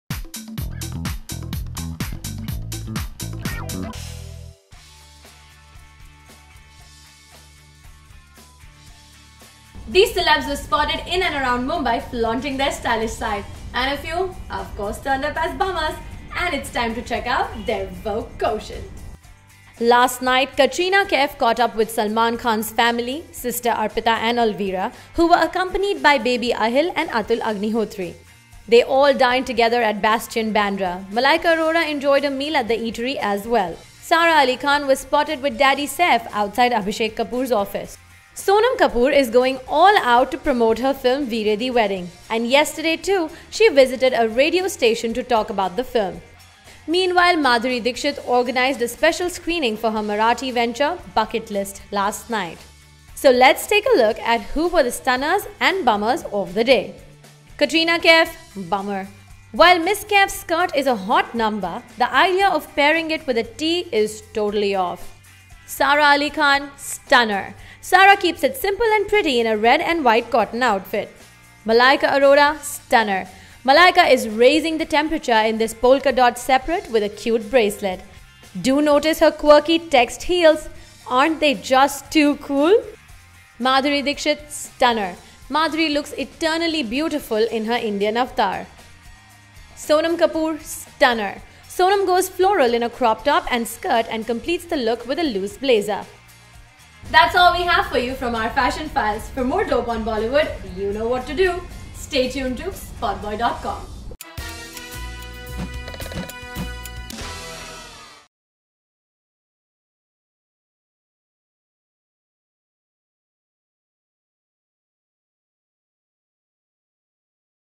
STUNNER OR BUMMER: Katrina Kaif, Sonam Kapoor, Sara Ali Khan, Malaika Arora Or Madhuri Dixit?
Bollywood ladies kept it stylish while a few turned out to be the bummers of the day. Here’s a check on these ladies’ fashion game...